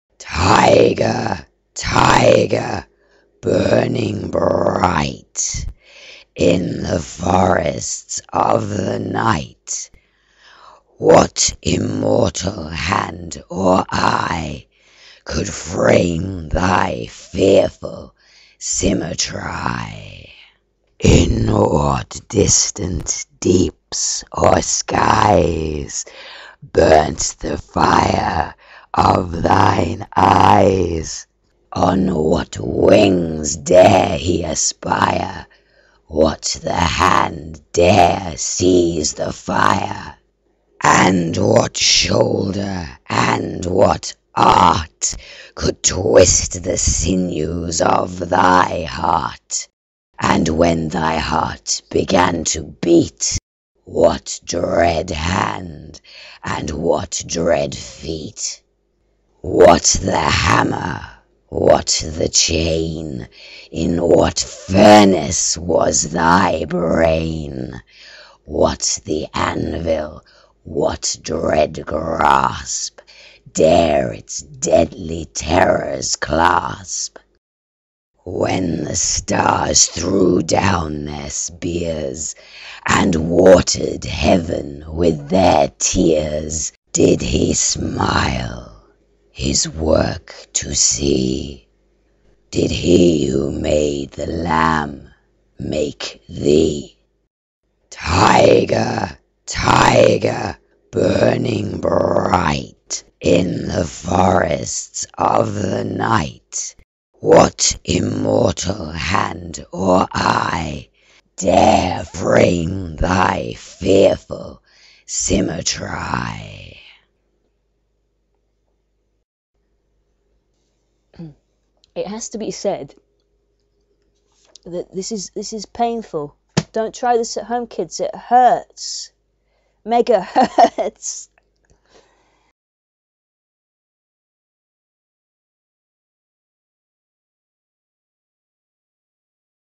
Pre-Interference Reading of William Blake's The Tyger (1794)
A reading of William Blake's famous poem.

THE TYGER (from Songs Of Experience)
By William Blake
Tyger! Tyger! burning bright
In the forests of the night,
What immortal hand or eye
Could frame thy fearful symmetry?

In what distant deeps or skies
Burnt the fire of thine eyes?
On what wings dare he aspire?
What the hand dare seize the fire?

And what shoulder, & what art,
Could twist the sinews of thy heart?
And when thy heart began to beat,
What dread hand? & what dread feet?

What the hammer? what the chain?
In what furnace was thy brain?
What the anvil? what dread grasp
Dare its deadly terrors clasp?

When the stars threw down their spears,
And watered heaven with their tears,
Did he smile his work to see?
Did he who made the Lamb make thee?

Dare frame thy fearful symmetry?

1794

The gag-able gag about Megahertz mega-hurting would o' been better as Killer-Hurts. (SIGH)